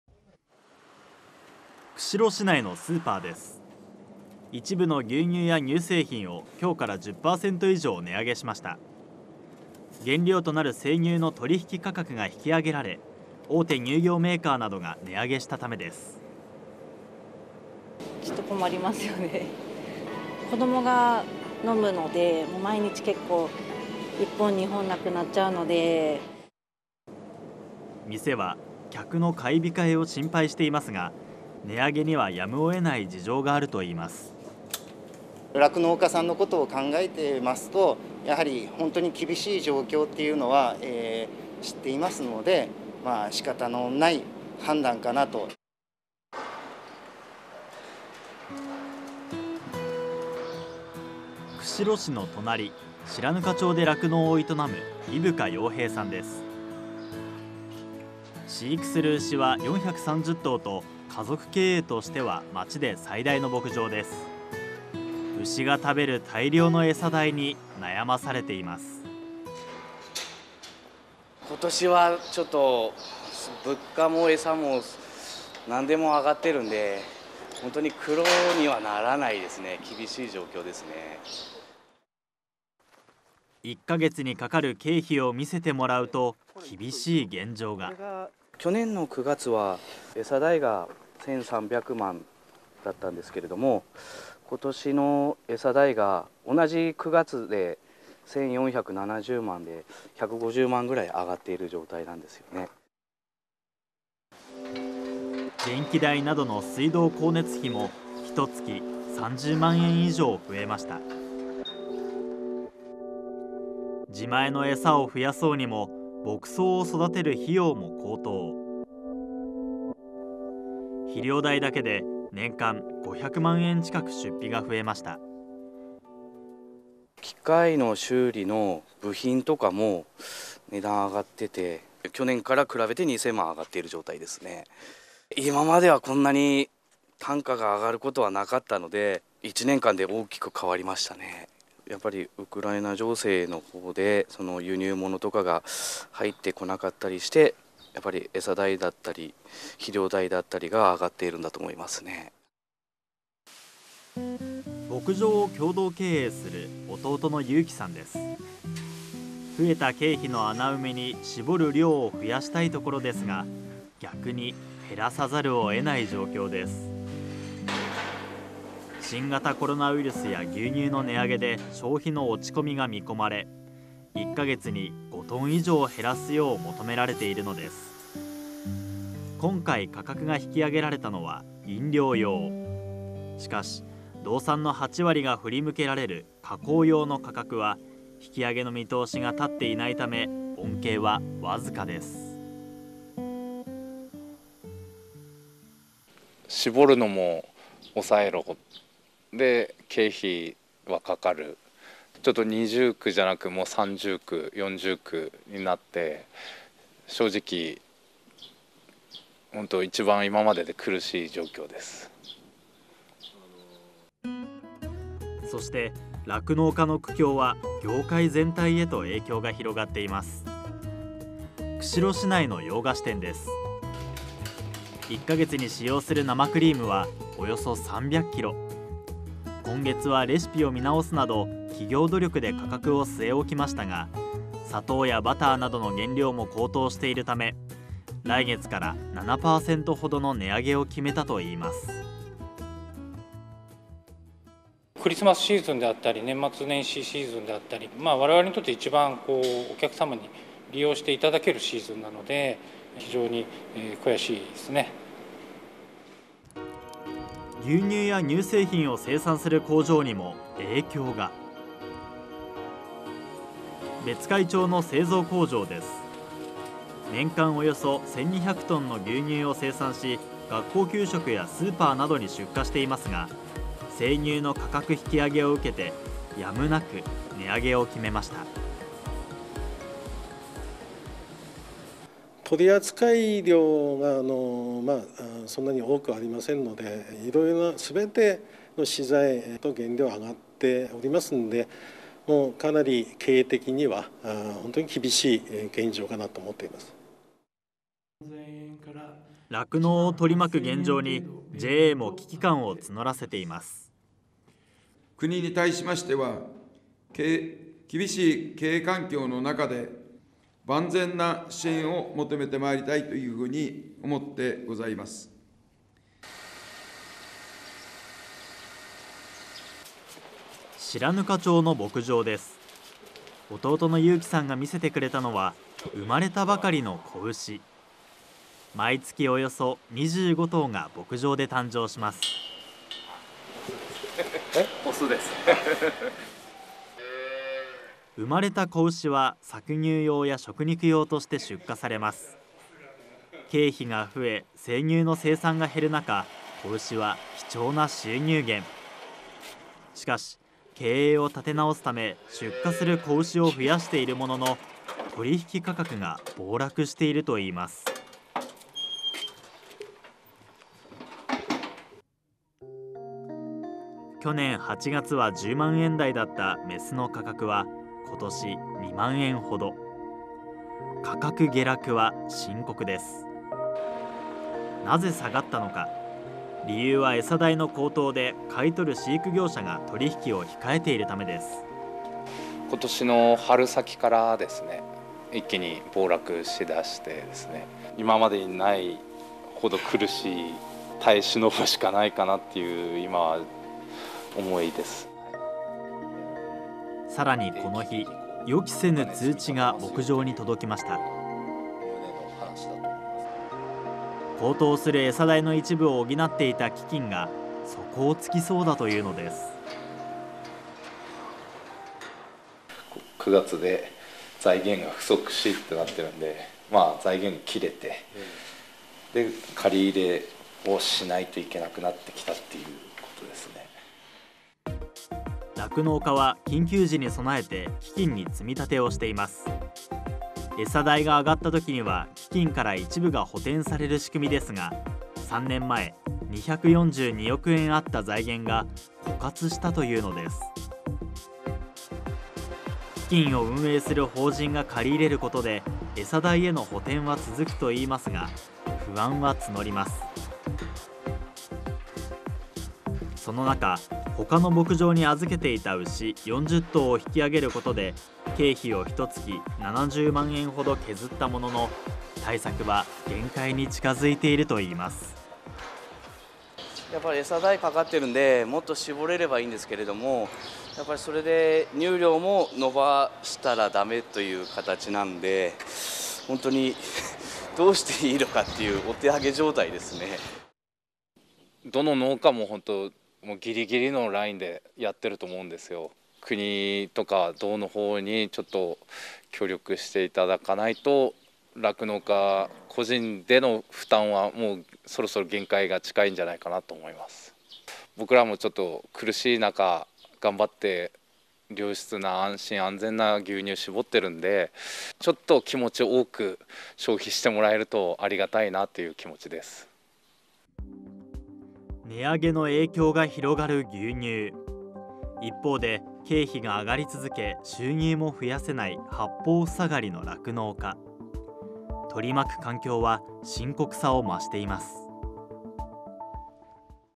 【牛乳も値上げ】酪農家に降りかかる経費…「八方ふさがり」で悲鳴
きょうから１１月。今月も値上げラッシュとなっていますが価格が上がったもののひとつに「牛乳」があります。背景には酪農家に降りかかる経費の増加や生産量を減らさざるを得ない「八方ふさがり」の状況があるといいます。いま酪農王国・北海道で何が起きているのでしょうか。（2022年11月1日放送）